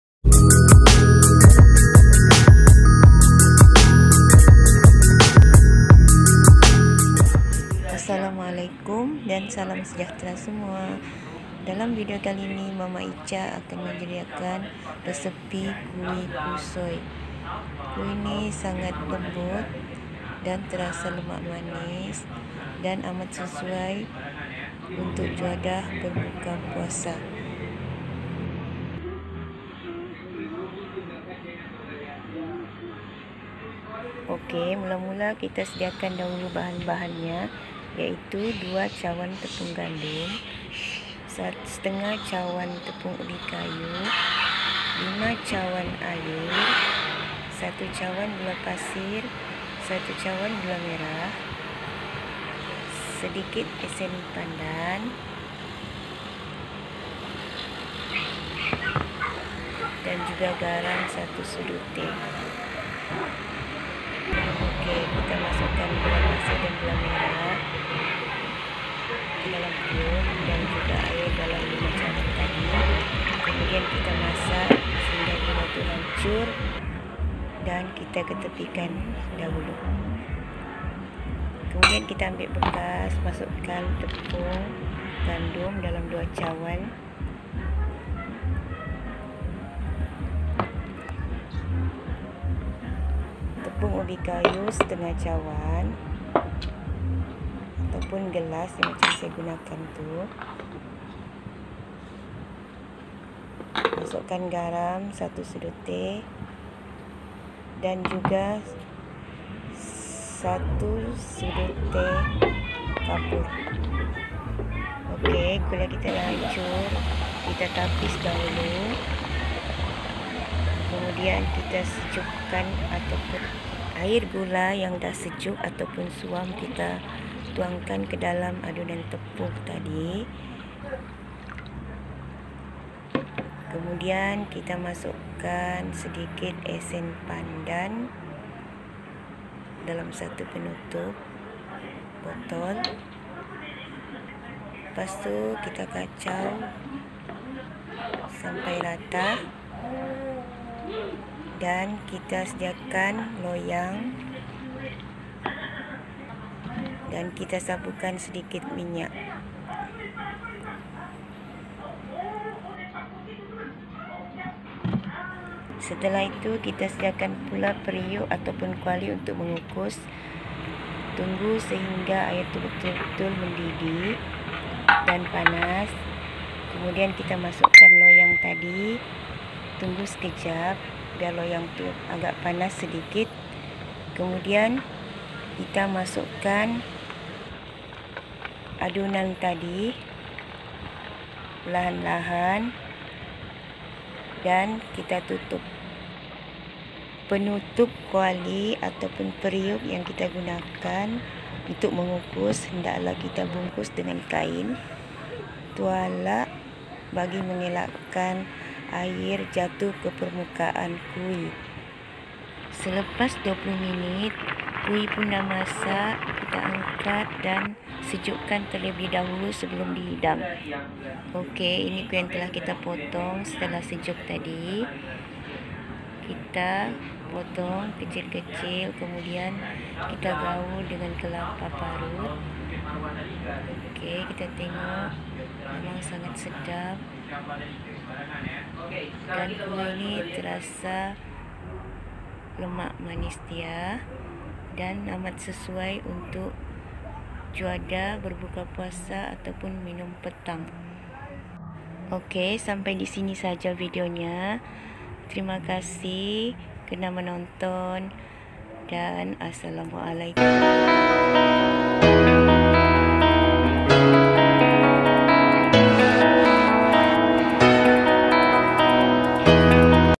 Kuih Kaswi Gula Merah sukatan cawan/gelas
Kuih Kaswi Gula Merah

Bahan-bahannya:

2 cwn Tepung Gandum
1/2 cwn Tepung Tapioca/kanji
1 cwn Gula merah
3/4 cwn gula pasir
5 cwn air tapis
1 sdt garam
1/2 sdt serbuk kapur
1 helai daun pandan/ esen pandan
1/2 biji kelapa parut (separuh tua)

Cara membuatnya sila tonton channel  video Dapur Mama Icha..
Selamat mencuba...💪👩‍🍳👍